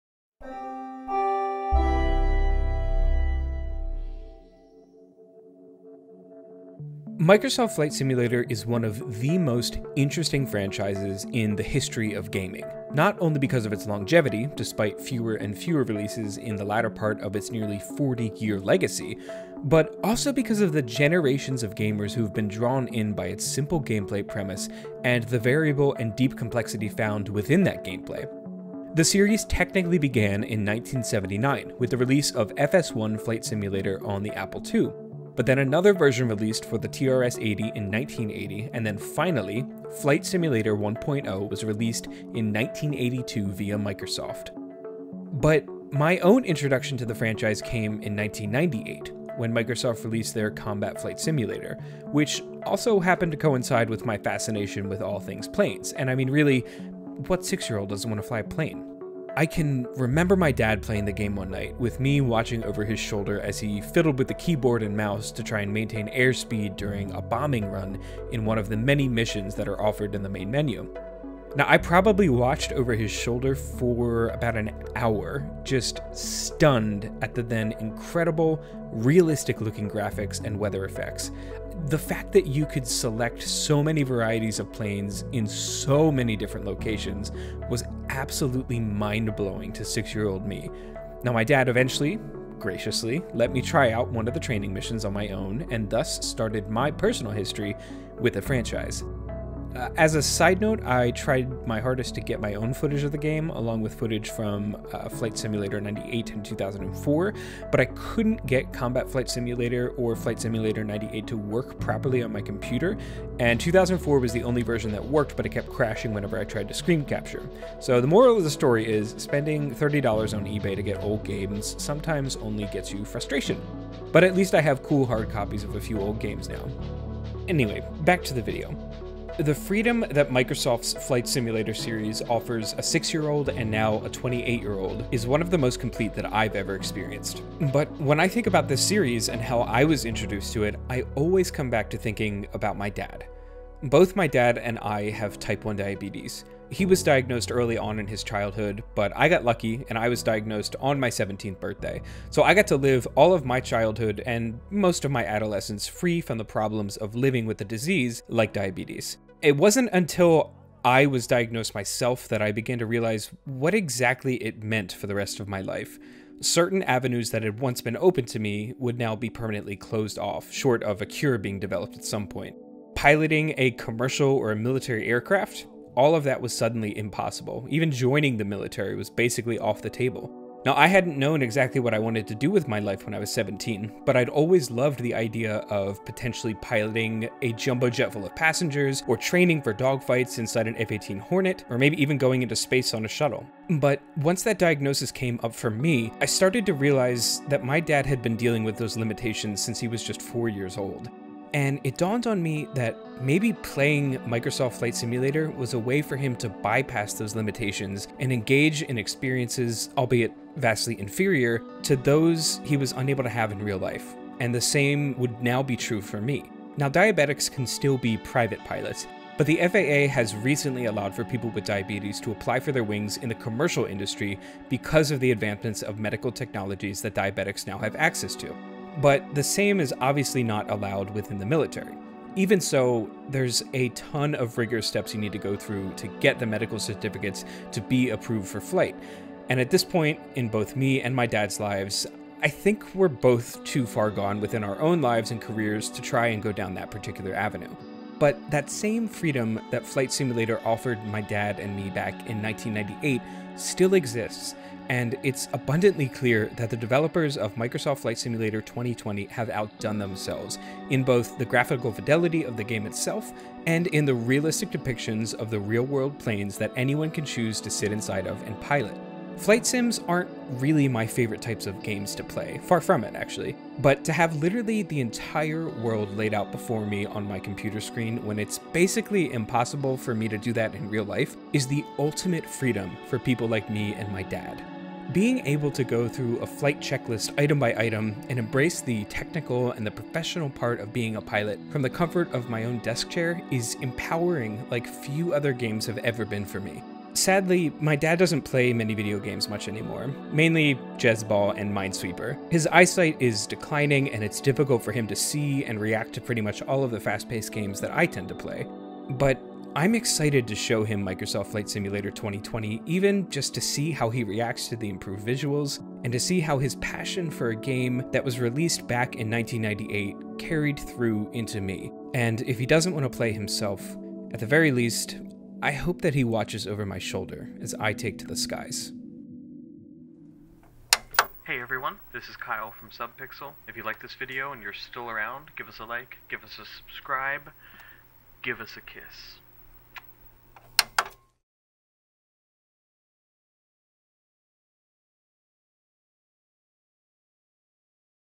The Ultimate Freedom of Microsoft Flight Simulator
Kyle goes deep into his history with the Microsoft Flight Simulator series and discusses its long and storied past, and how formative it was to him growing up. Also, diabetes sucks.